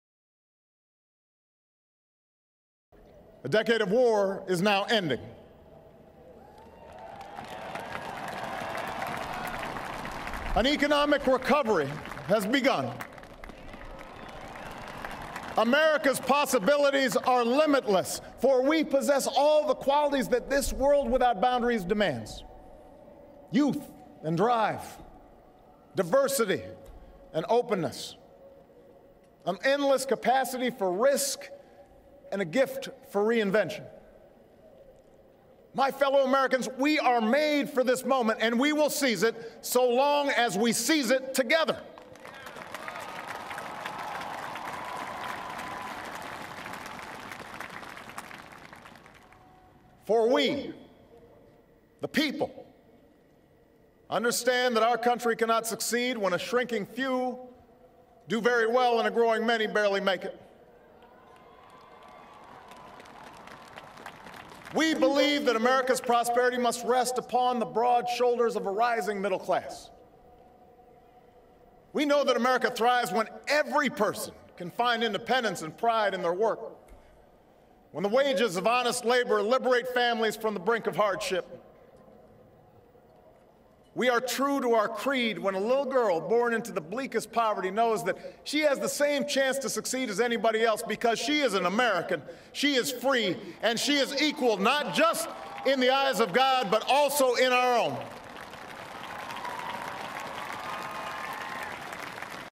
President Obama: 'America's possibilities are endless'
President Obama tells the thousands of people gathered in Washington that a decade of war is now ending and an economic recovery has begun.